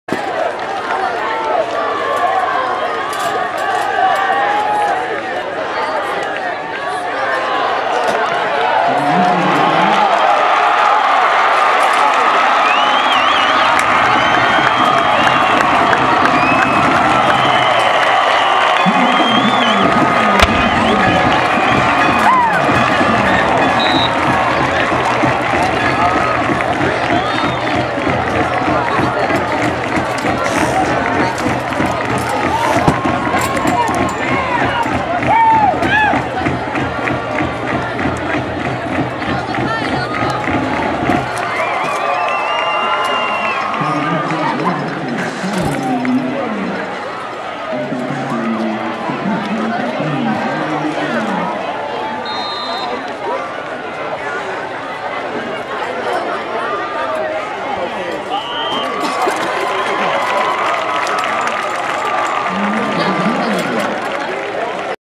West Valley @ Shasta high school football 9-2-11: Shasta's first TD
Shasta's Cody Lunsford scores on a 70 yd run in the first quarter to give Shasta an early lead 7-3.